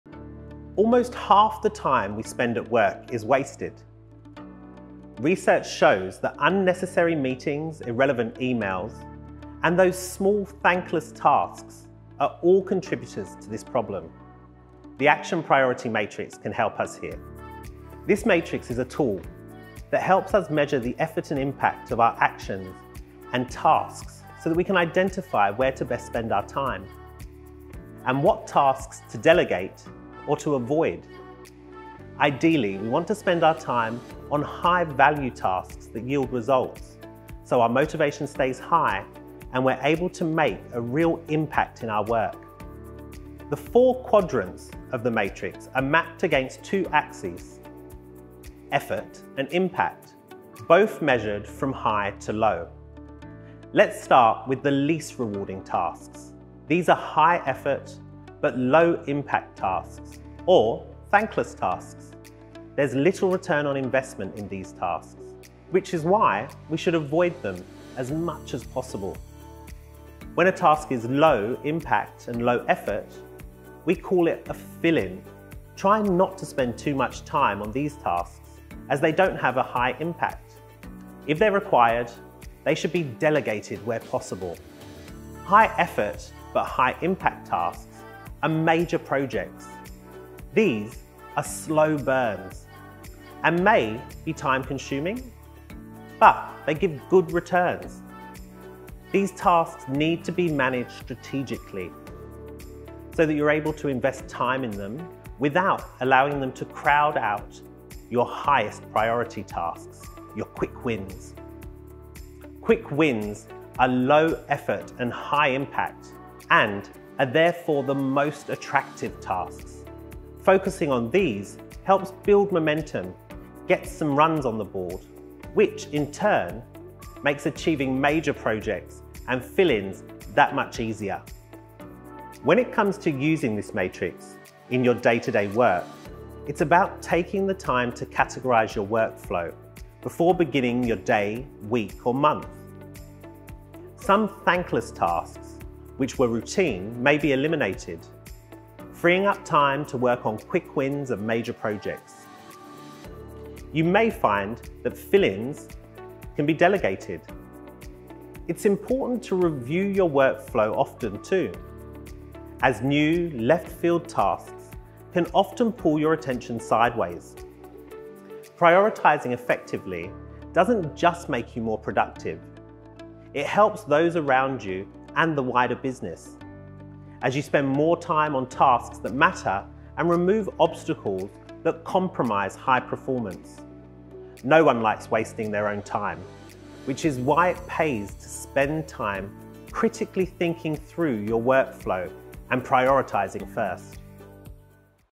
How to prioritise better (Using the 'Action/Impact Matrix')
'The 'Action/Priority Matrix' helps us prioritise high value tasks and delegate or delete others, so we're putting our energy into the right things.

In this video, we explore the following:
0:14 – Overview of the 'Action/Priority Matrix'
0:42 – Understanding the four quadrants of the 'Action/Priority Matrix'
2:11 – Putting the 'Action/Priority Matrix' to work on the job